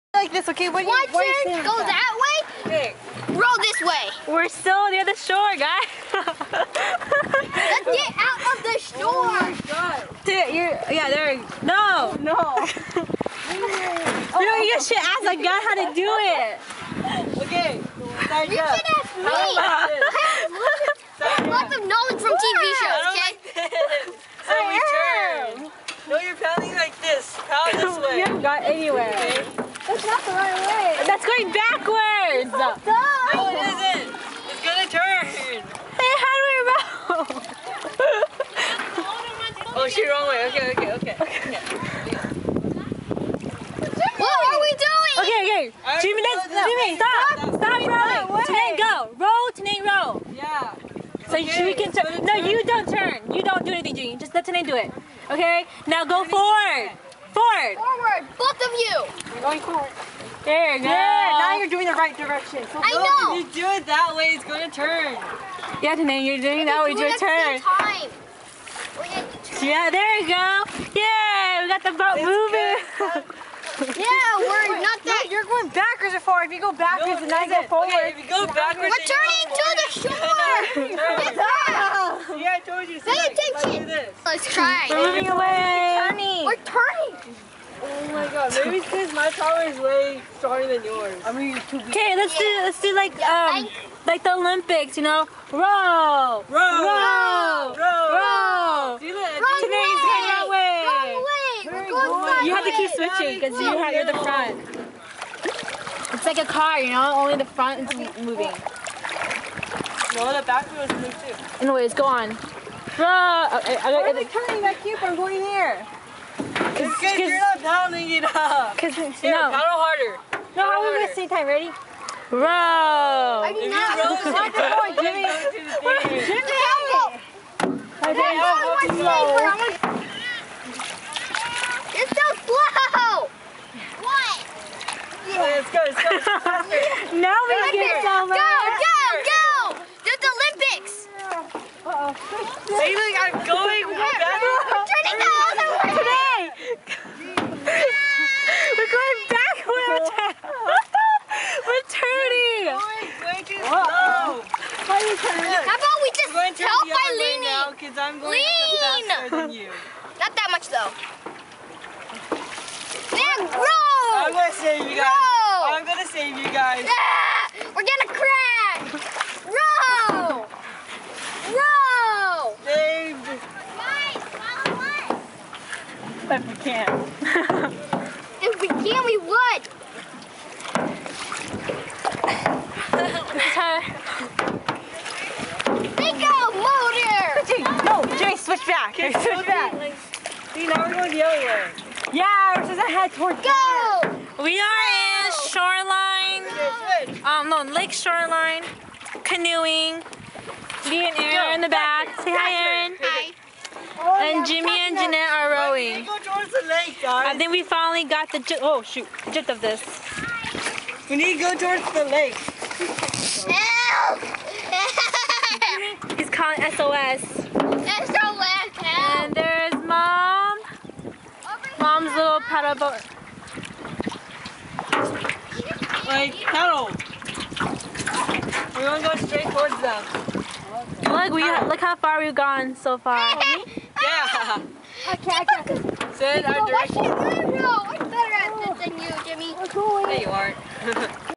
Canoeing with fam
We kayaked, canoed, and paddled. Yay. mm...in this video, it starts off with an argument on how to row..hahaha. it was our first time :] hehe ..but we're smart and got the hang of it at the end

VIEW ON HD for best viewing!